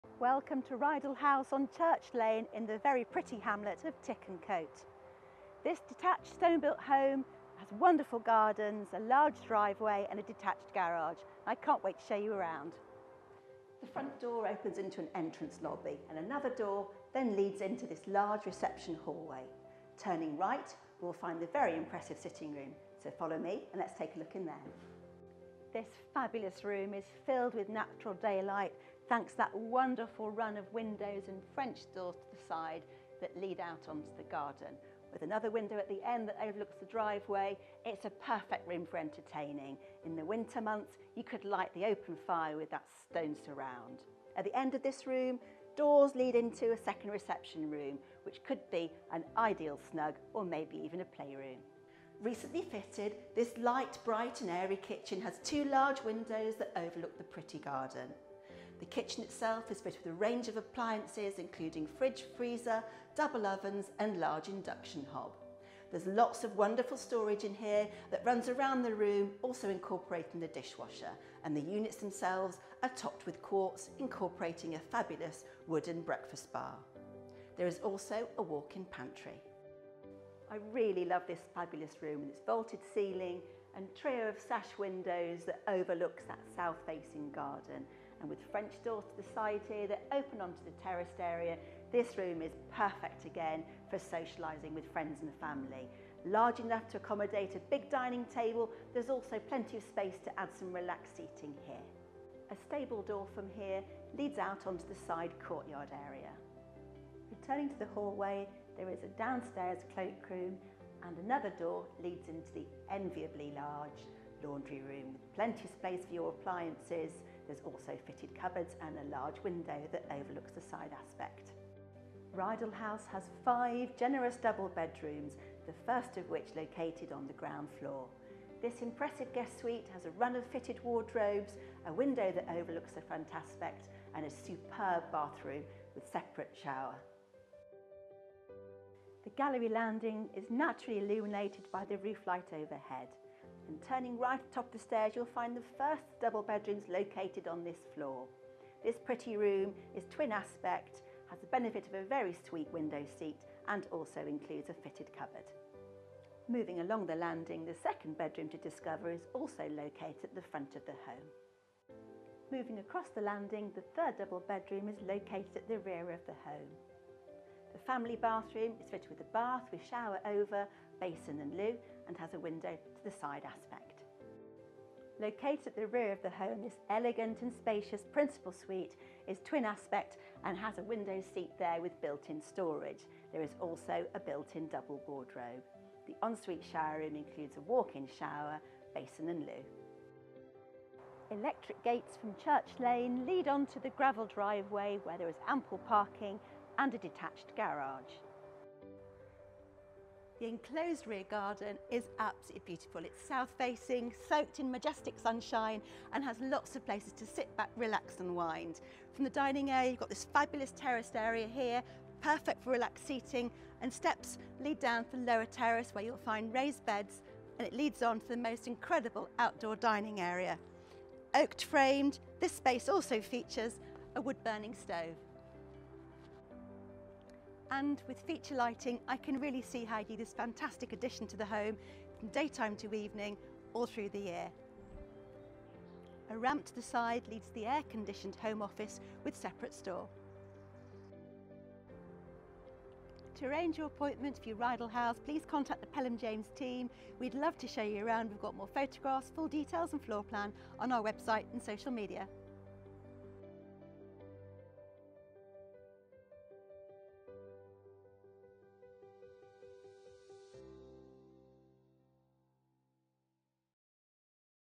Rydal House, Tickencote - with Lottie Crooke, Pelham James Unique Homes
Under offer...

Welcome to Rydal House, a harmonious five bedroom family home with great flow, in the historic hamlet of Tickencote. Set off leafy Church Lane, a quintessential country lane,  tree-lined and bordered by dry stone walls and deep stone kerbs.

We'd love to show you around - call the PJ team on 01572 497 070 to arrange your viewing.